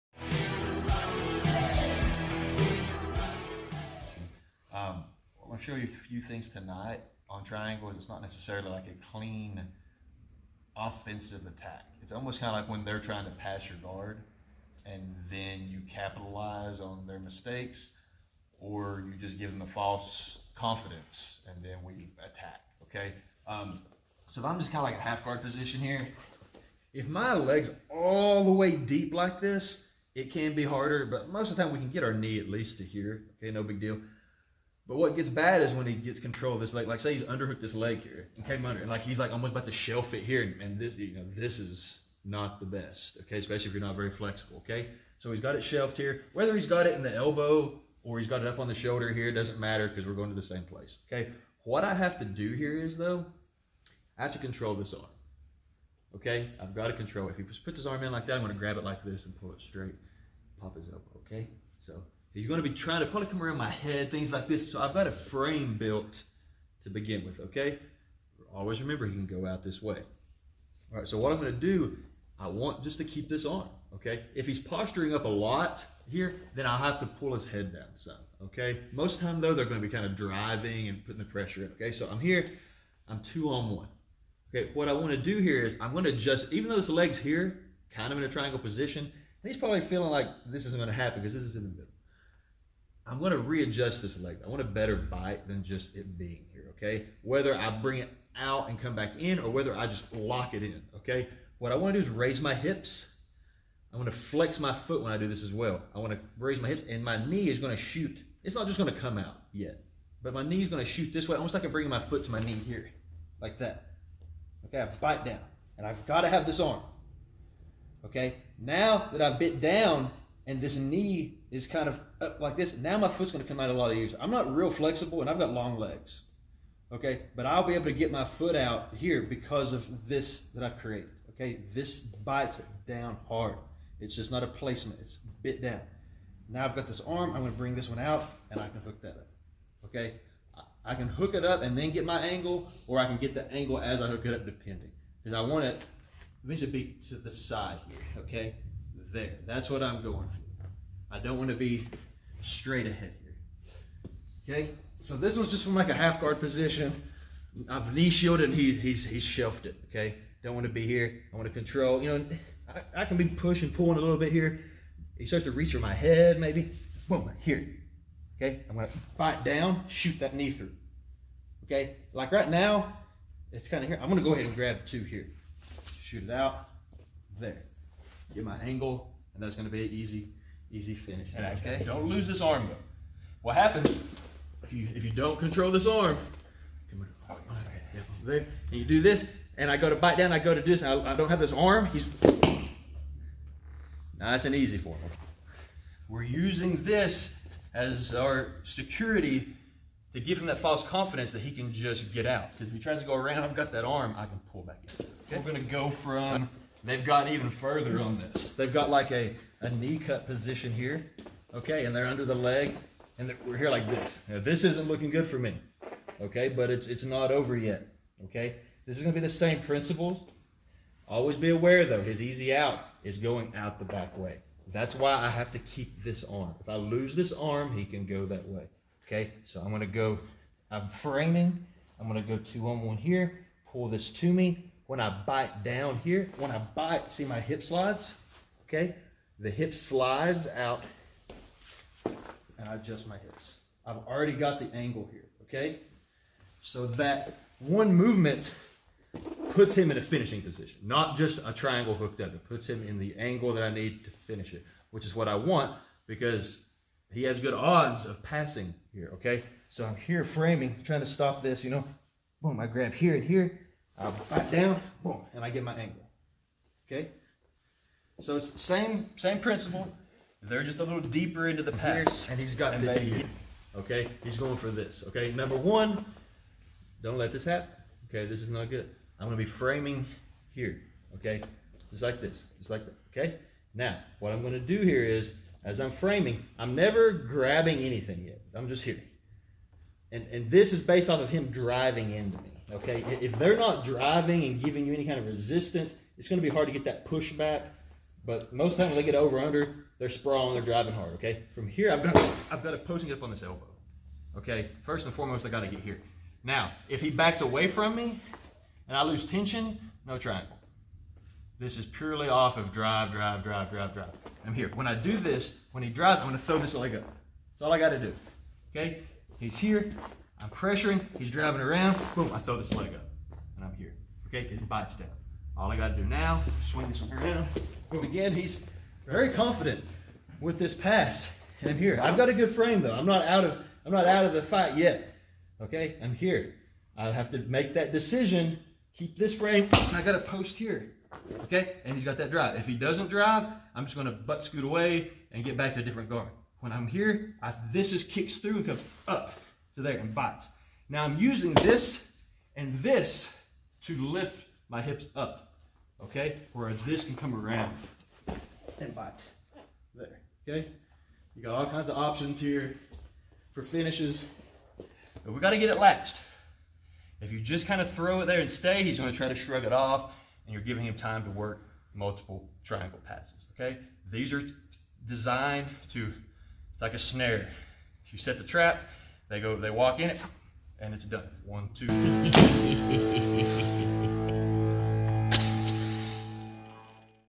3 Triangle Attacks (when they think they are passing your guard)
I have always been a big fan of triangles and this is 2 sneaky ways to suprise your opponent while they think they are about to pass your guard.
If you have been a fan of Ryan Hall these may seem familiar🤙